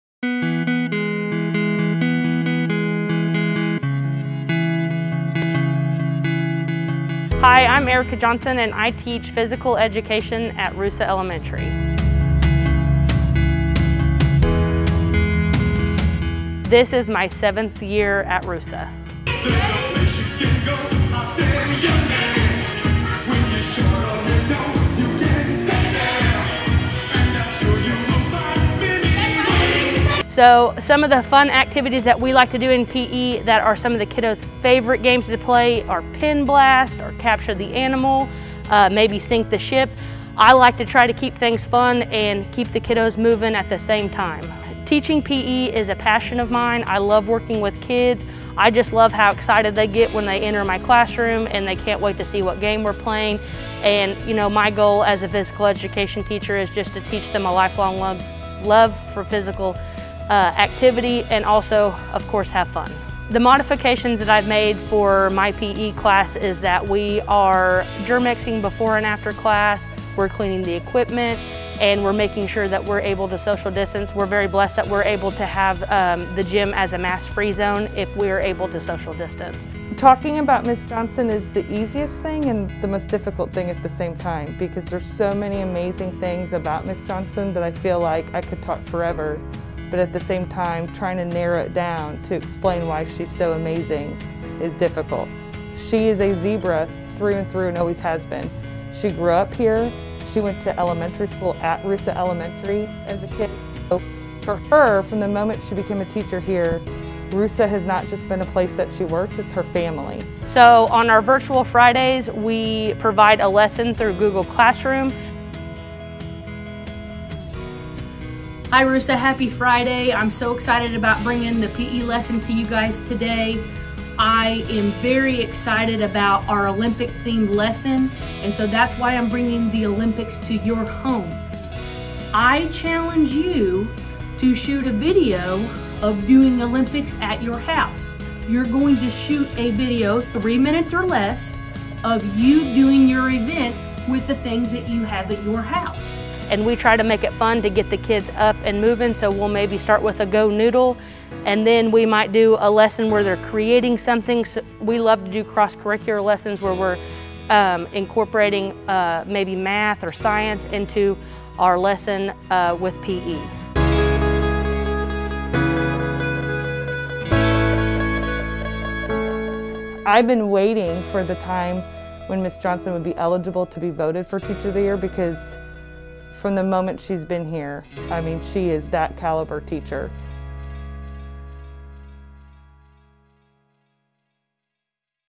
Stuart Roosa Elementary Teacher of the Year
Erica Johnson is the physical education teacher at Roosa Elementary.  She loves being a Claremore Zebra where she graduated high school.

Ms. Johnson is loved and respected by all of her colleagues and students.  She enjoys keeping her students active and teaching them how to be healthy.

Please welcome our Roosa Elementary Teacher of the Year: Erica Johnson!